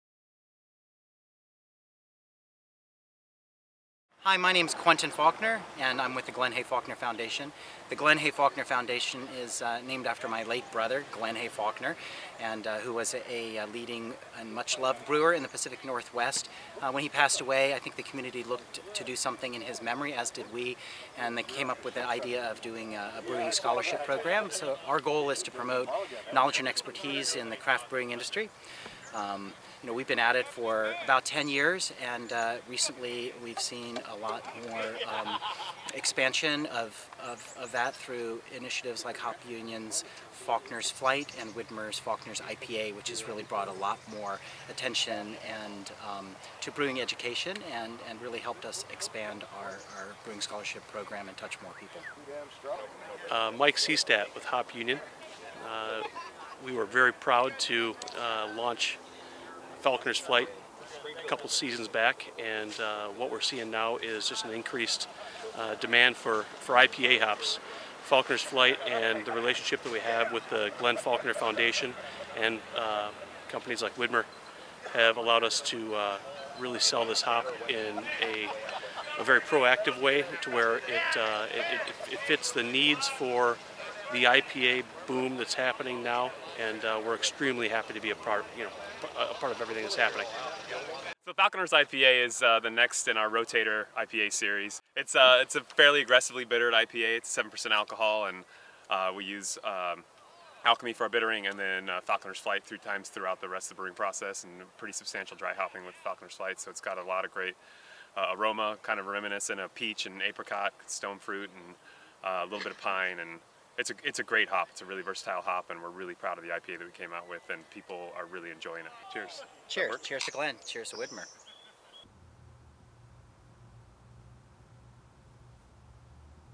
Rotator IPA Series: Falconer's IPA
Quentin Falconer of the Glenn Hay Falconer Foundation, Mike Seestadt of Hop Union and Jacob Leonard from Widmer Brother Brewing share the story behind Falconer's IPA and its unique hops.